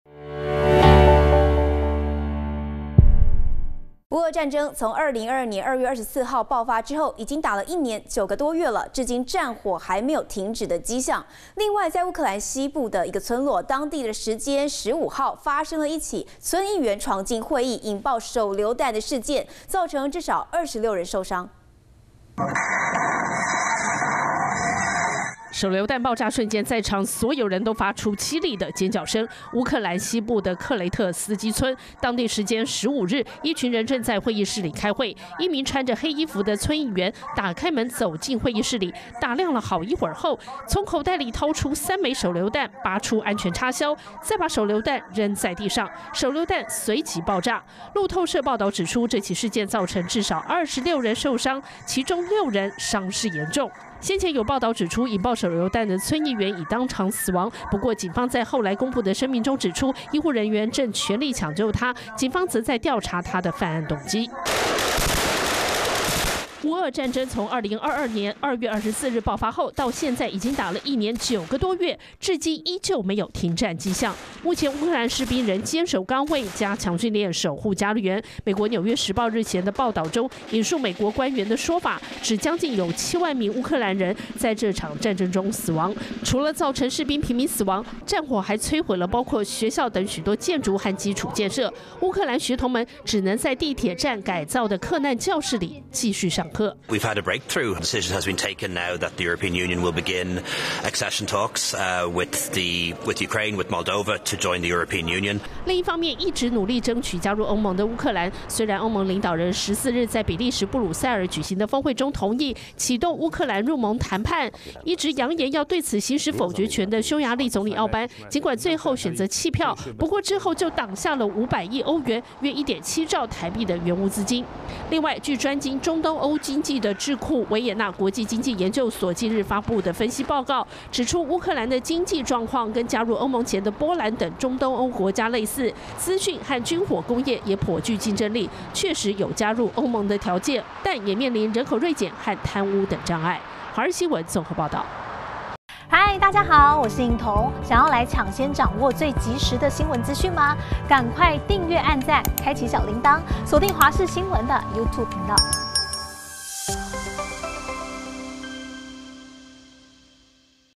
烏克蘭村議員闖議場「突扔3手榴彈」 至少26人受傷｜華視新聞 20231216
烏俄戰爭從2022年2月24日爆發後已經打了1年9個多月，至今戰火仍然沒有停止跡象。另外烏克蘭西部的克雷特斯基村當地時間15日，則發生了一起村議員闖入會議引爆手榴彈的事件，造成至少26人受傷。